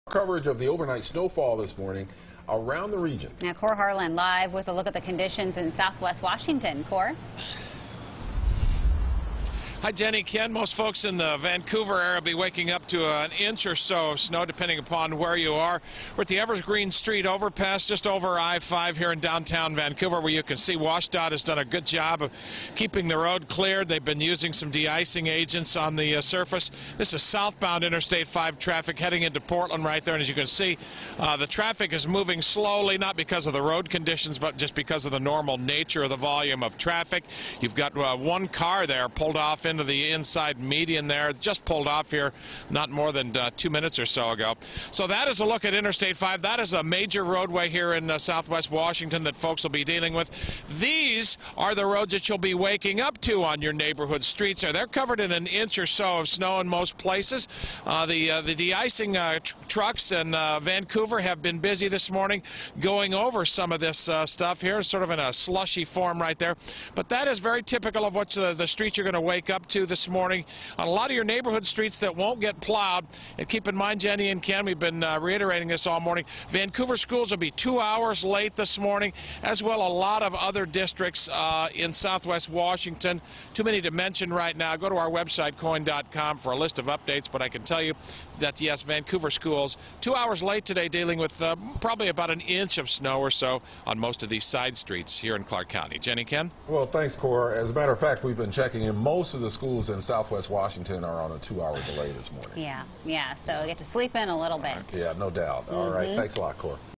Overnight snow impacting morning commute in Vancouver
The highways seem to be clean of any snow, so it's the side streets people will have to cautious around as they head out Tuesday morning.